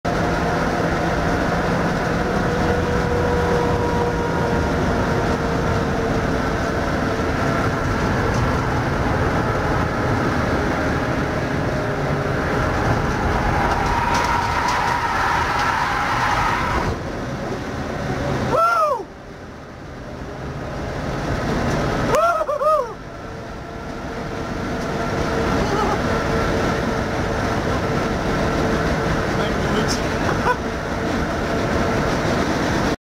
EnduroKA Cadwell Park Heart in mouth moment
Massive slide as the rear let go whilst chasing down 2 other cars at Cadwell.

Made some strange noises immediately after.
1. Impressed with my own skill - can't believe i did that
2. Can't believe i actually survived that
3. I think I've s..t my pants